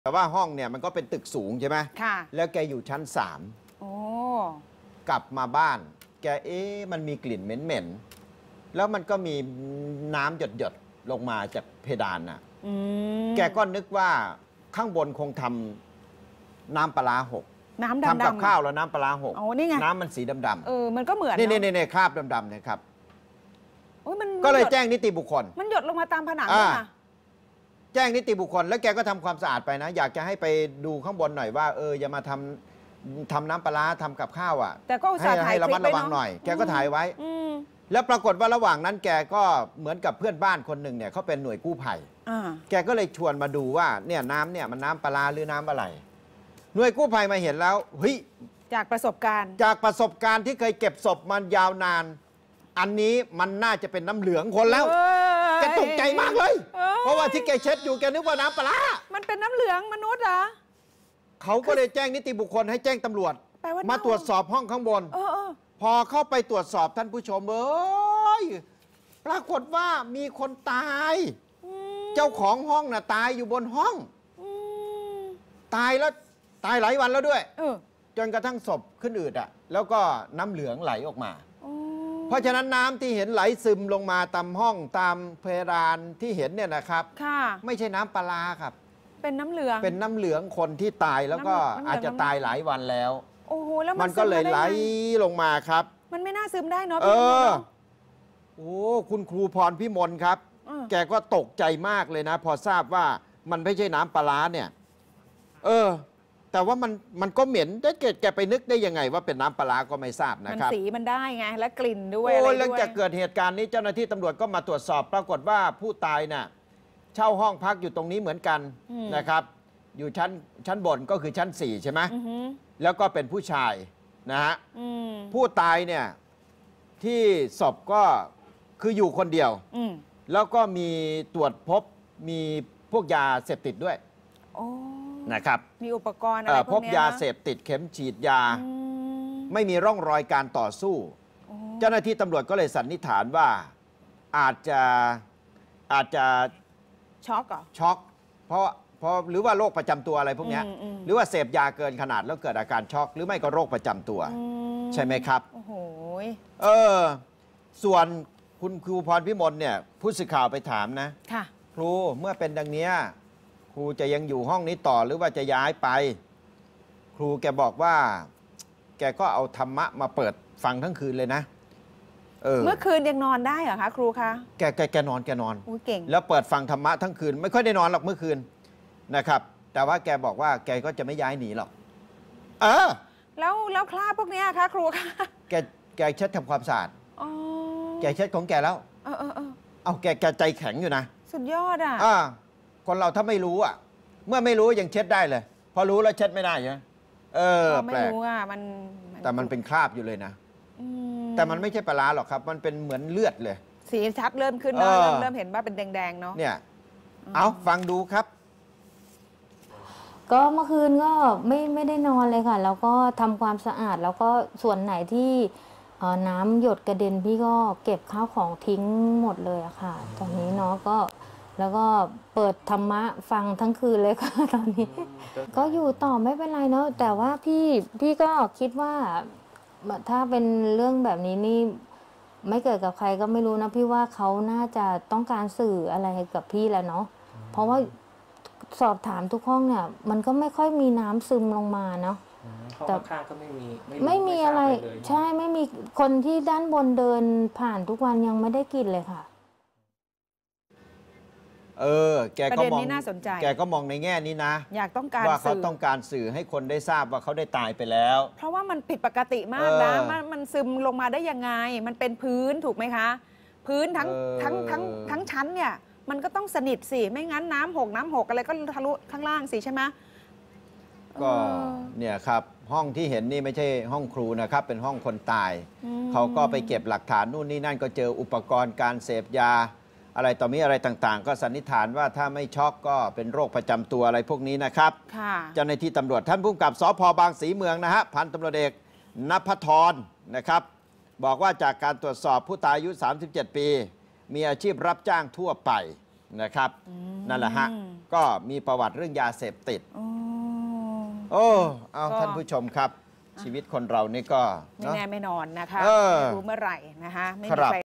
สยอง! คิดว่าน้ำปลาร้ารั่ว สุดท้ายเป็นน้ำเหลือง
ครูเช่าห้องอยู่ชั้น 3 เห็นน้ำสีดำมีกลิ่นเหม็นซึมลงมาจากเพดานหยดลงพื้น คิดว่าชั้นบนทำน้ำปลาร้าหก ที่ไหนได้เป็นน้ำจากศพผู้เช่าชั้นบน คุณครูสุดหลอน เช็ดถูห้องเสร็จ ฟังธรรมะข่มจิตใจ บอกว่ายังอยู่ที่นี่ต่อไป